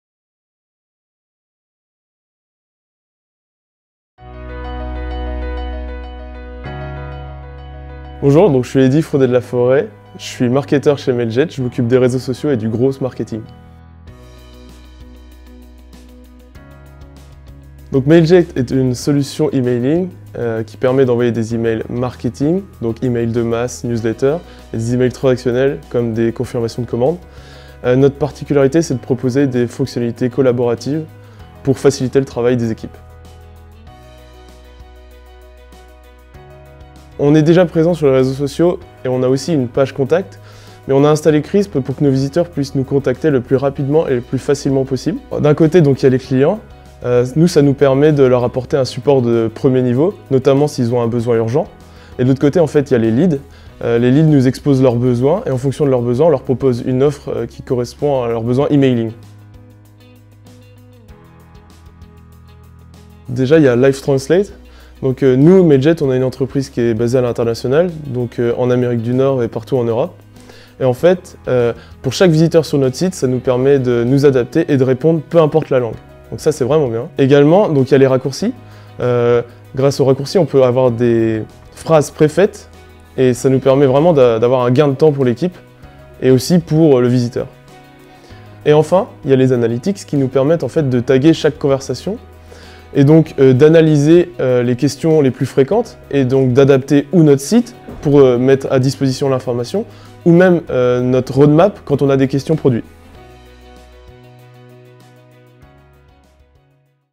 How Mailjet use Crisp to better support their customers & leads relationship
Through this user story, Mailjet explain how they use Crisp to enhance their customer relationship thanks to Live Chat.

Thanks to Live Chat, they better support their customers questions and are able to get more leads with Crisp.

The main feature they use :
- Live Translate
- Shortcuts
- Analytics